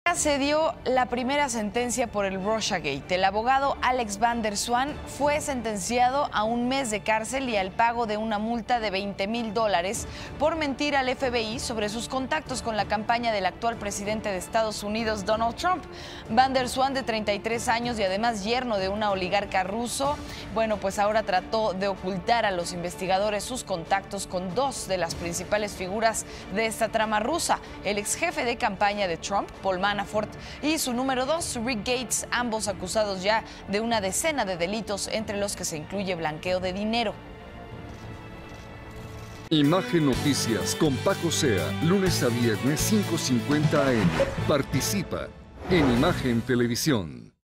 Dictan la primera sentencia por el 'Rusiagate' | Noticias con Francisco Zea
¡Dictan la primera  sentencia por el 'Rusiagate'! El abogado Alex Van Der Zwaan es condenado a un mes de cárcel y al pago de una multa por mentir al FBI.